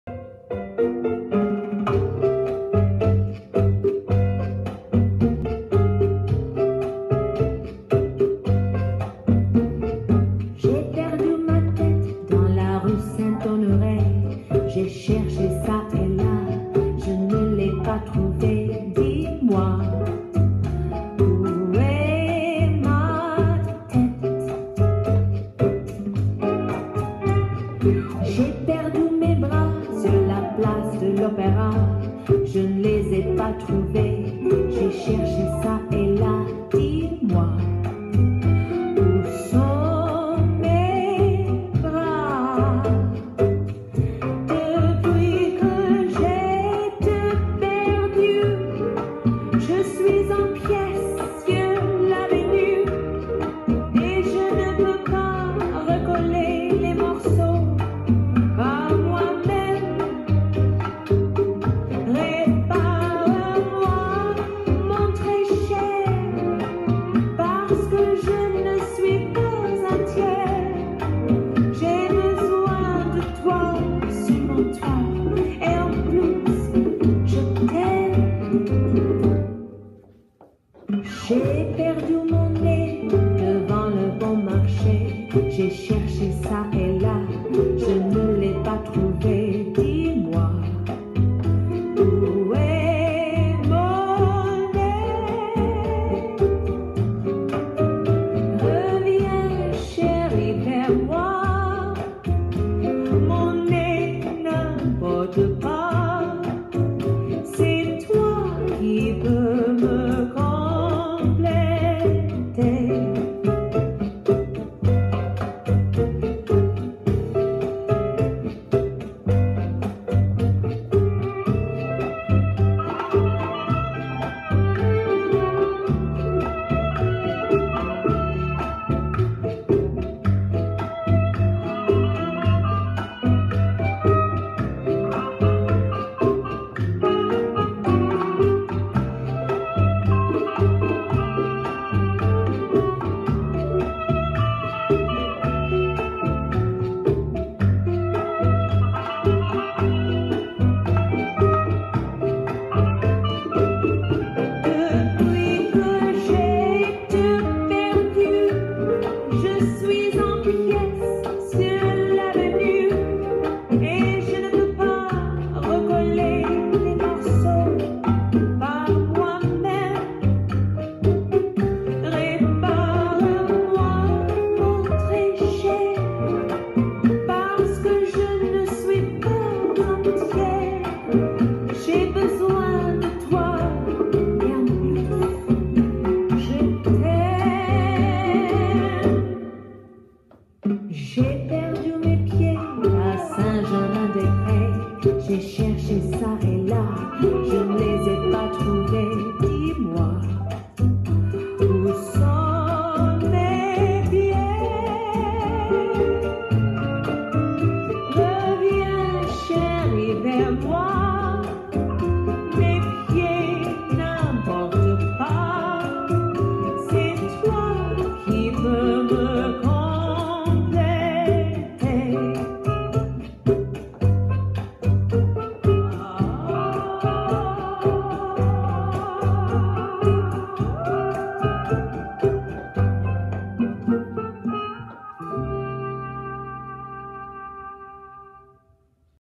Bossa Nouveau Line Dance
32 count 4 wall
One Restart on end of wall 6, facing 6 o’clock.